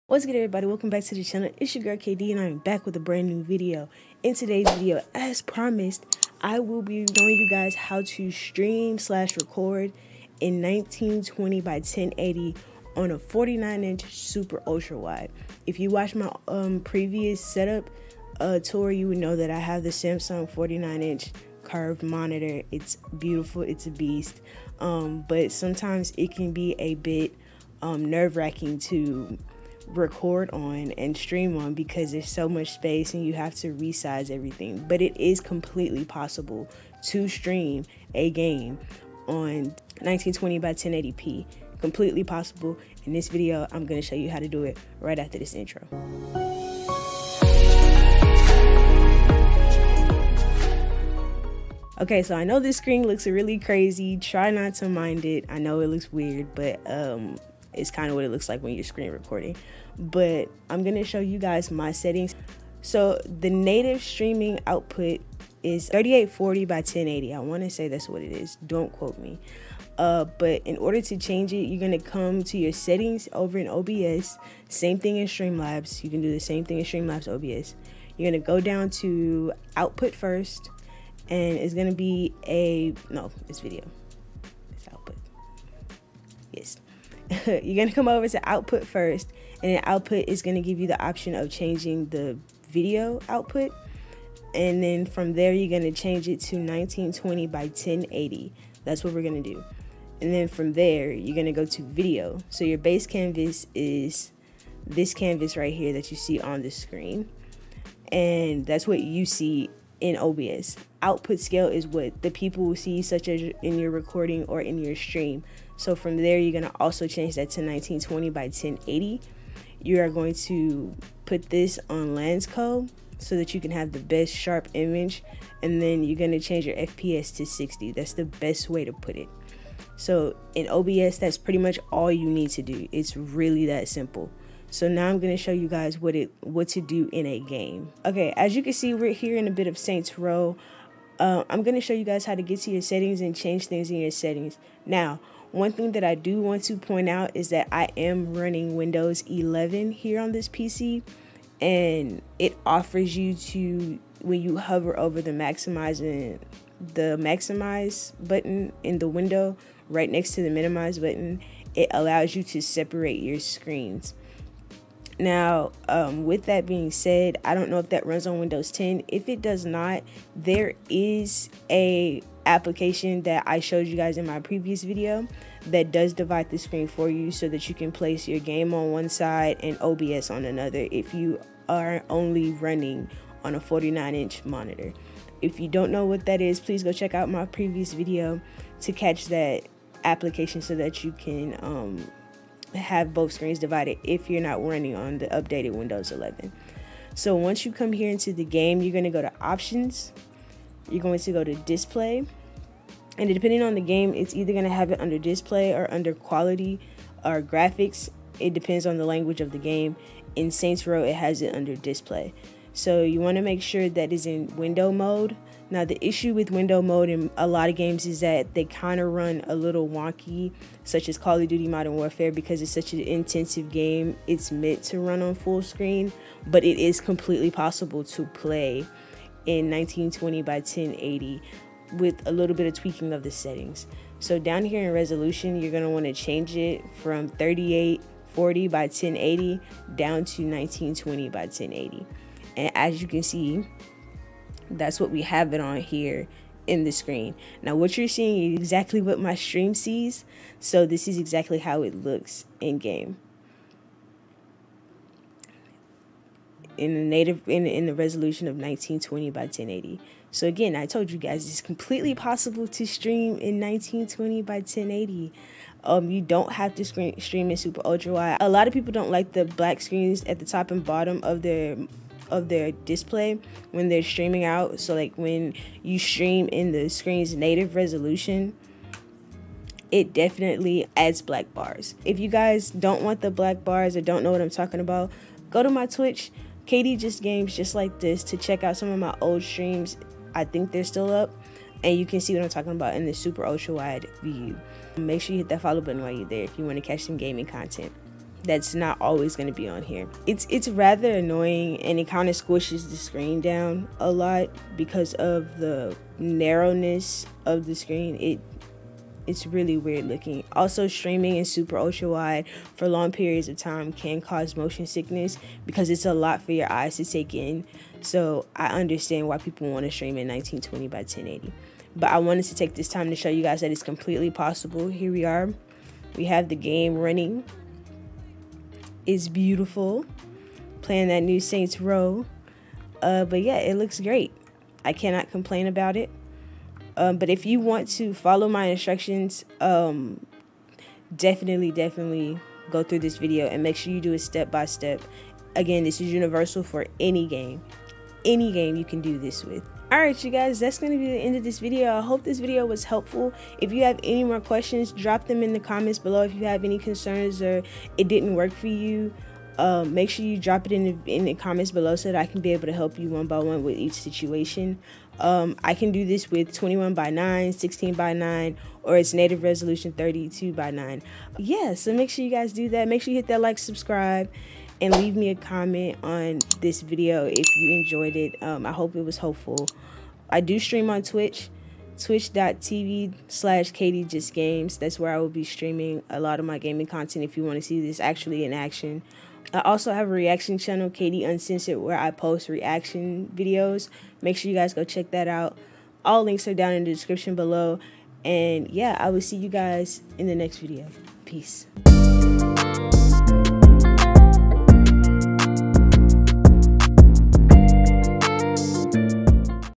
How To Record in 16:9 on a SUPER ULTRAWIDE
In today's Video, I will show you how to setup a super ultra wide montior to record and stream in 1920 by 1080.